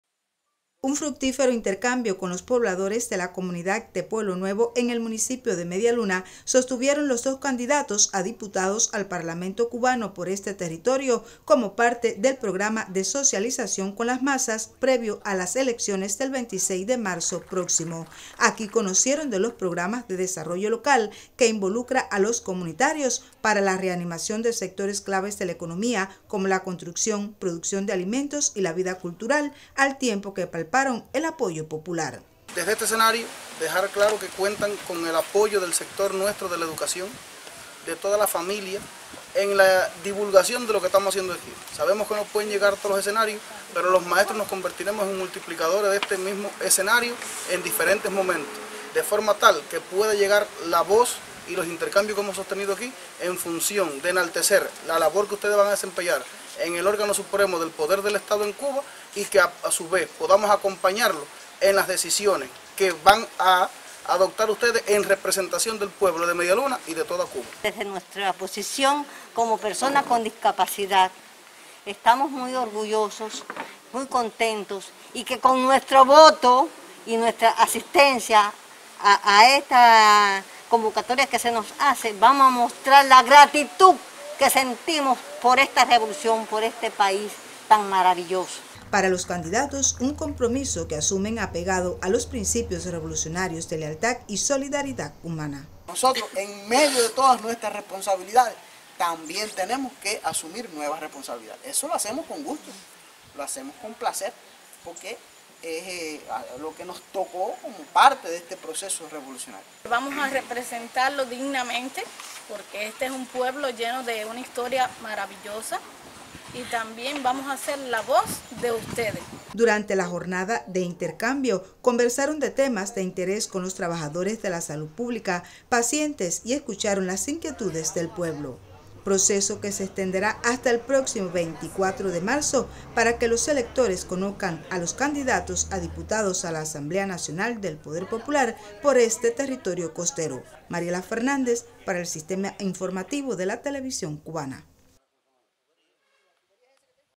Candidatos a diputados al parlamento cubano por el Municipio de Media Luna intercambian con el puebl
Los dos candidatos a diputados al parlamento por el municipio de Media Luna Yanisey López Reyes, directora de la secundaria básica Elías Zaldiva Álvarez, y Aníbal Ernesto Ramos Socarras, rector de la Universidad de Ciencias Médicas de Granma, intercambiaron con medialuneros en centros laborales y comunidades.